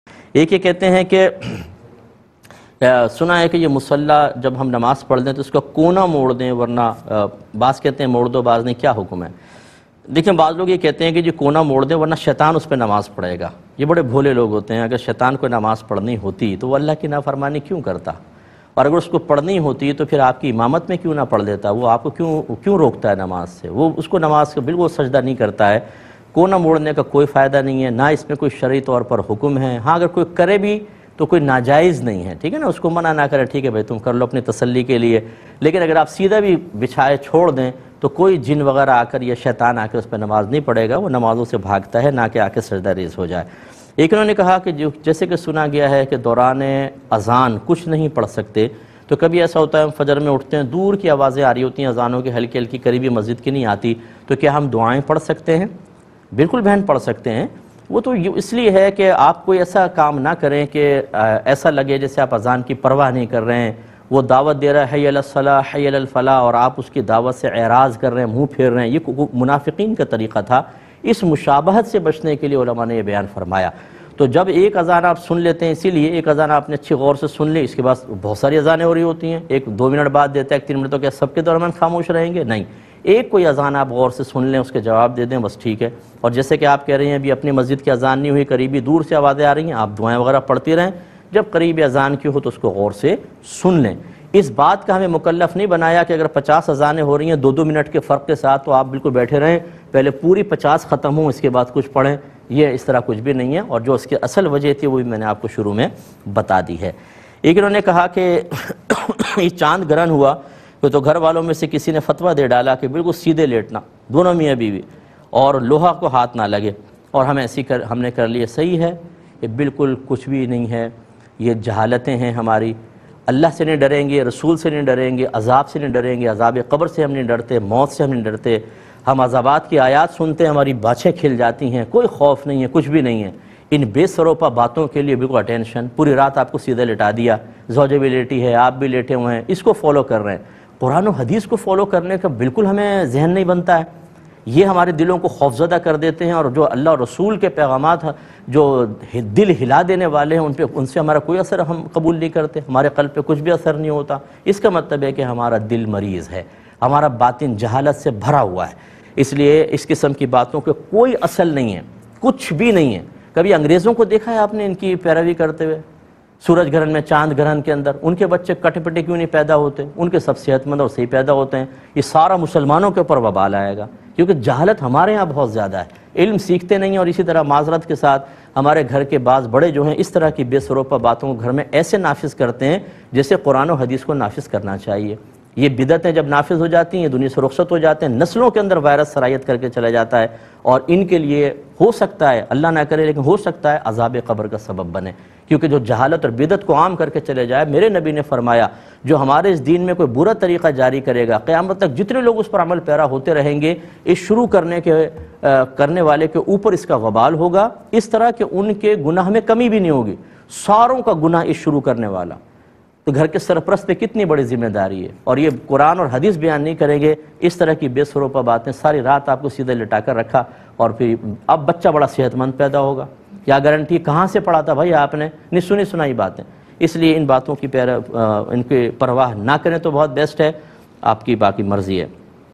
JaNamaz ka Kona Fold Na Kare To Shaitan Namaz Parhta Hai? | Mufti Muhammad Akmal | ARY Qtv
JaNamaz ka Kona Fold Na Kya To Shaitan Namaz Parhta Hai? | JaNamaz Fold Kyun Ki Jaati Hai